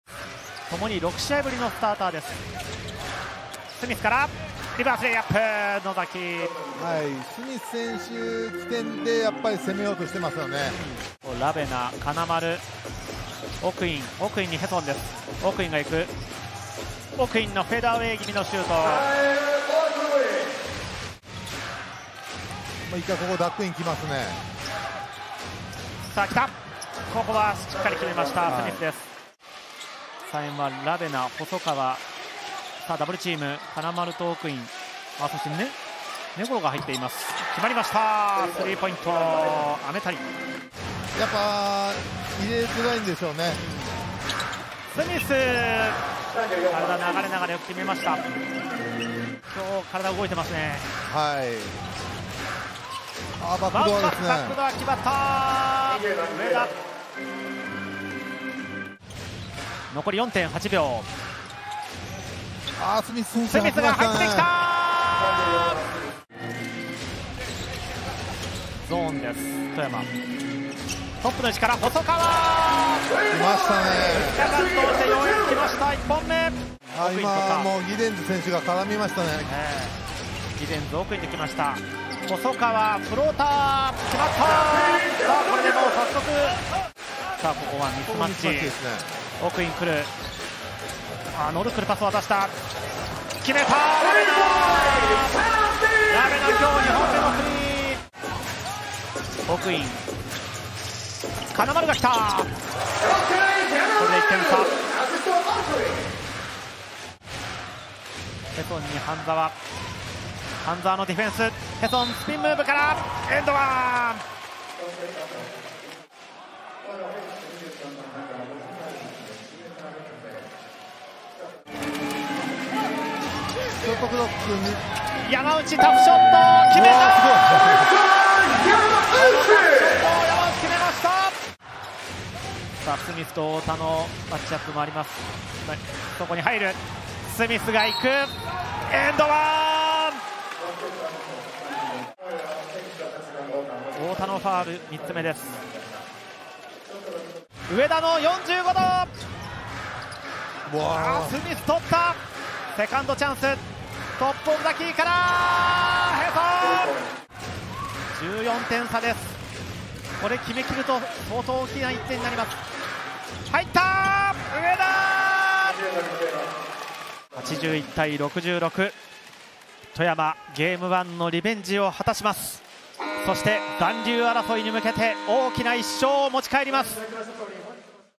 【ハイライト】三遠ネオフェニックスvs富山グラウジーズ｜B1 第28節 GAME2｜04.02.2023 プロバスケ (Bリーグ)
Watch the Game Highlights from San-En Neophoenix vs. Toyama Grouses, 04/02/2023